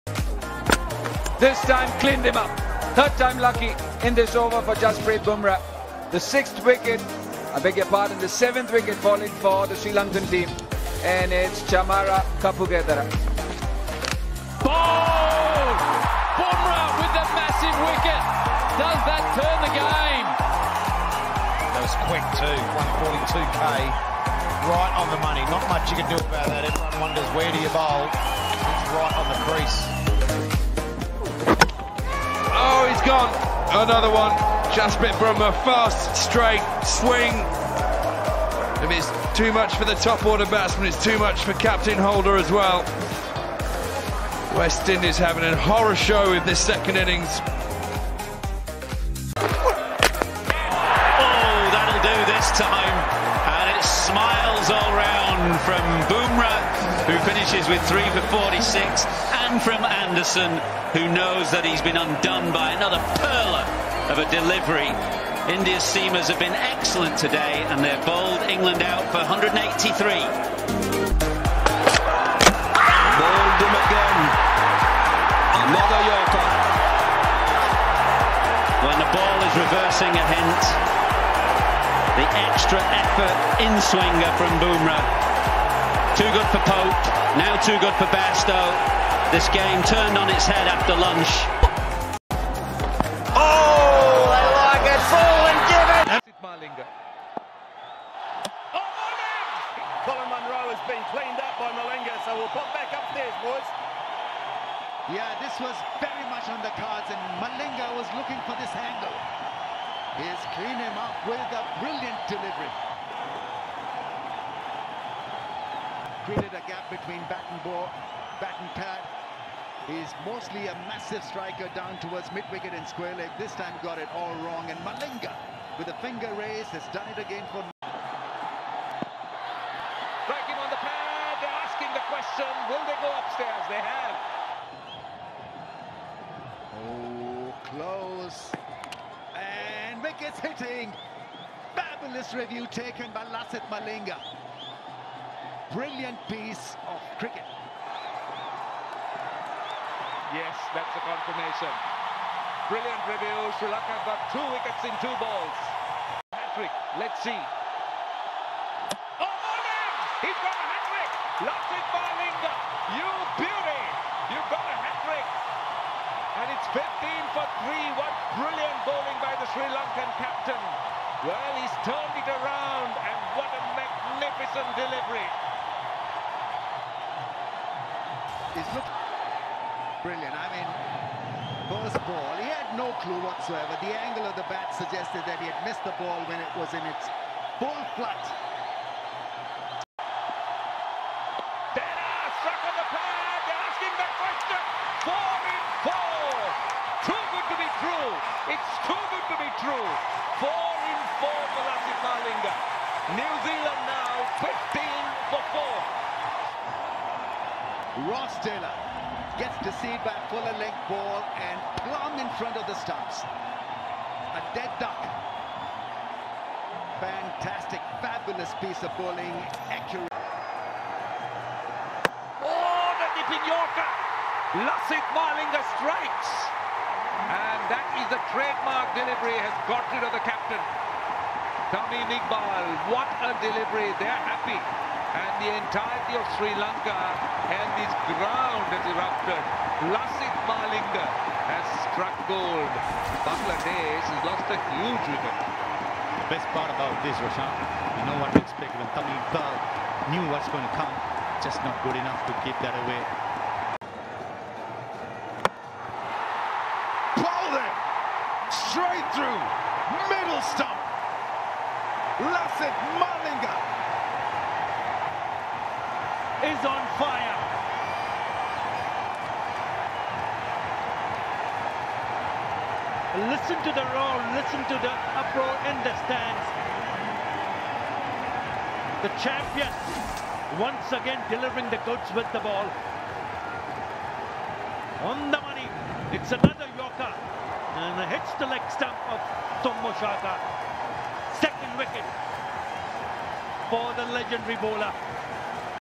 Best Yorker Bowler? Lasith Malinga | Mitchell Starc | Jasprit Bumrah
Hello guys in this video you can see a comparison between Bumra and Malinga.There was a copyright on Mitchell Stark part so I had to remove the Starc part. So sorry about that guys. But I think you can still enjoy the video.